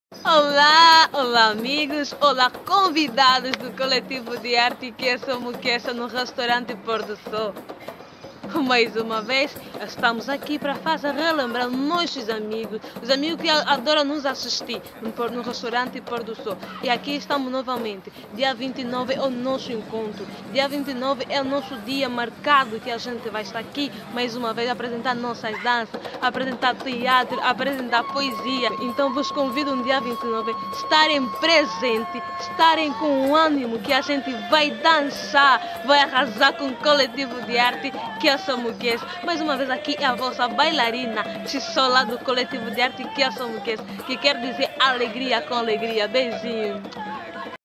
29 09 Kiese Mu Kiese no Por do Sol
No domingo, dia 29 de Setembro de 2019 você tem mais um ENCONTRO MARCADO com a arte e cultura tradicional angolana no Pôr do Sol - Complexo Turístico, preconizado pelo Colectivo de Artes Kiese Mu Kiese que, em Kikongo significa ALEGRIA COM ALEGRIA. Enquanto se diverte saboreia uma das melhores culinárias oferecidas em Angola / África, com pratos da cozinha nacional e internacional.